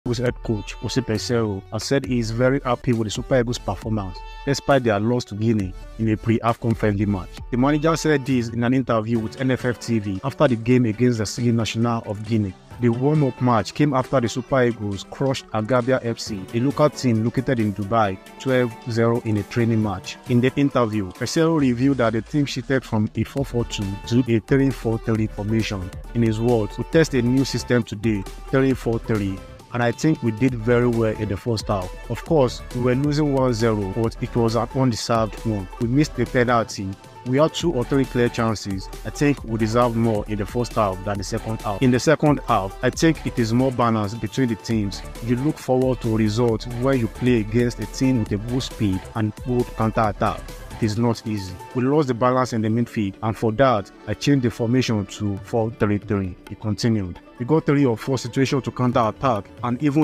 Peseiro is "HAPPY" with the Super Eagles after thier loss to Guinea#supereagles
In a candid interview, Coach Jose Peseiro shares his thoughts on the Super Eagles’ display in a friendly against Guinea. Despite a loss, Peseiro expressed satisfaction with the team’s performance, especially after experimenting with a shift from a 4-4-2 to a 3-4-3 formation. He highlighted missed opportunities, a penalty, and strategic changes that affected the game’s outcome. Managing the team’s dynamics, Peseiro noted his contentment with the overall squad’s performance, expressing optimism for the upcoming AFCON tournament.